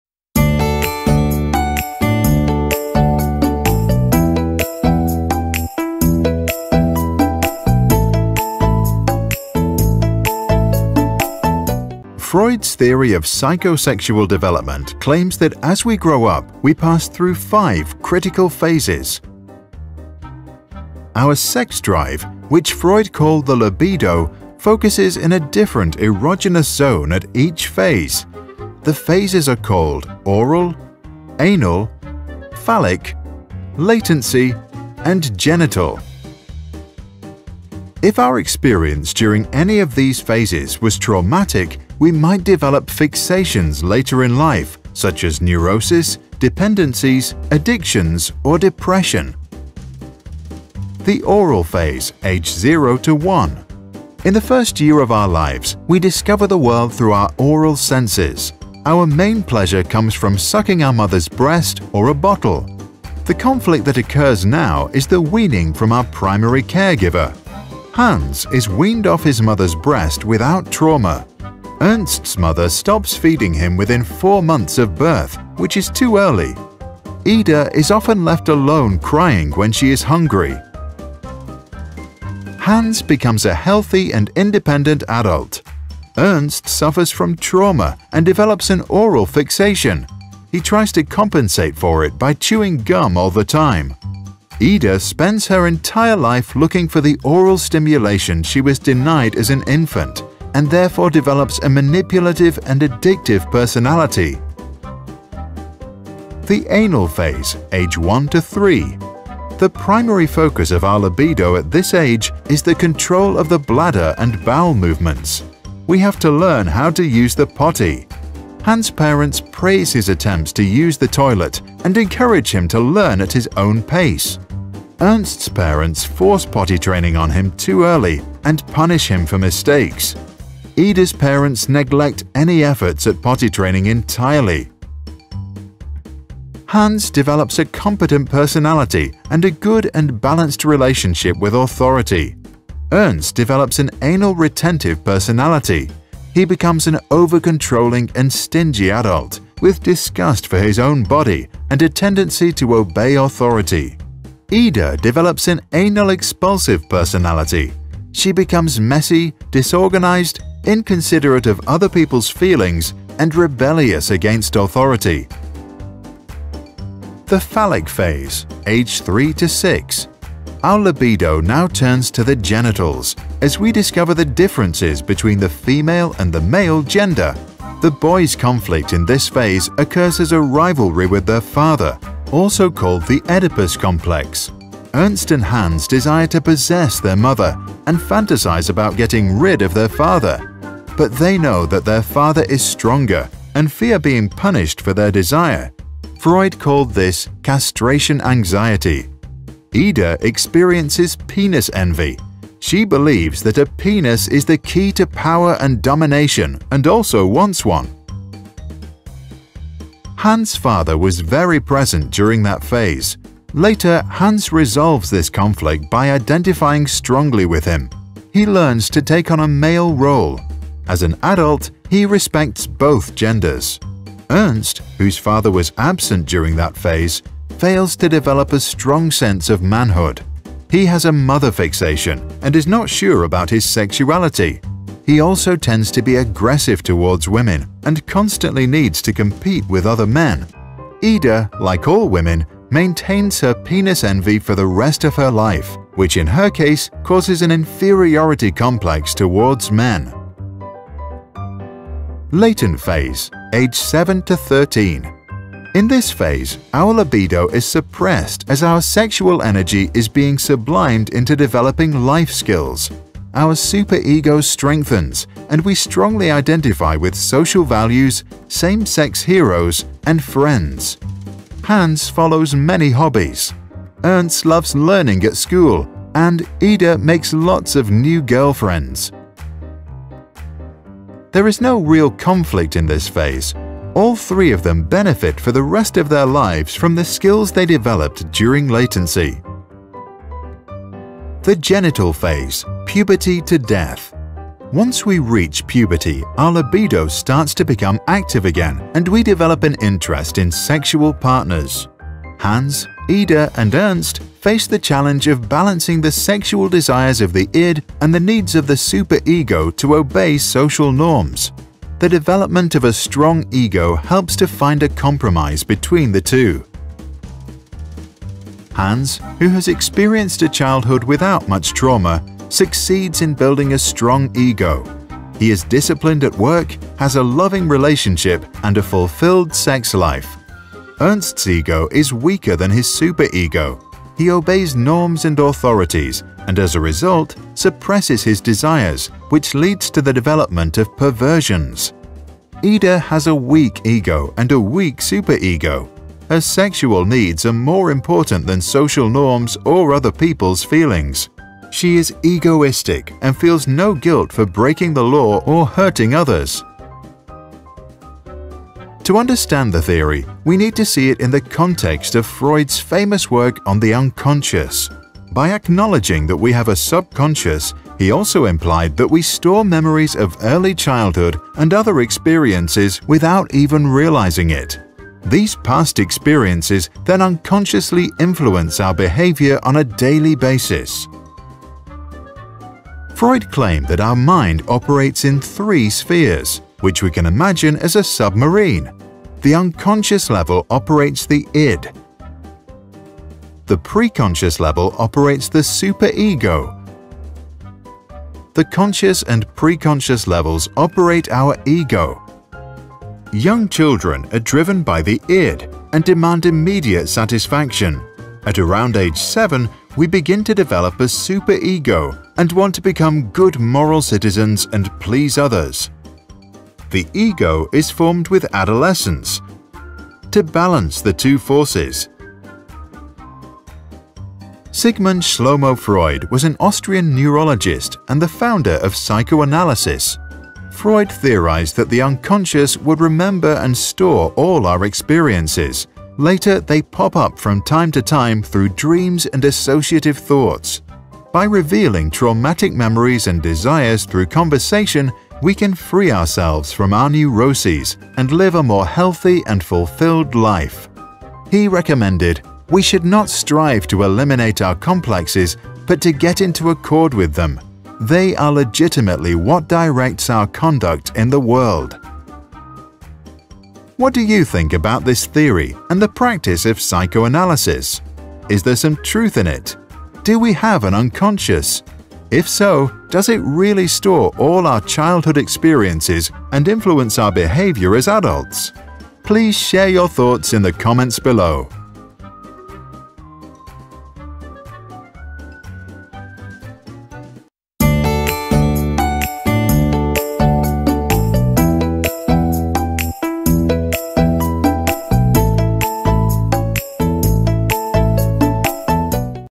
Freuds Theory of Psychosexual Development
Freud’s theory of psychosexual development claims that as we grow up, we pass through five critical phases which are defined by our sexual drive, also called libido, concentrating at specific erogenous zones.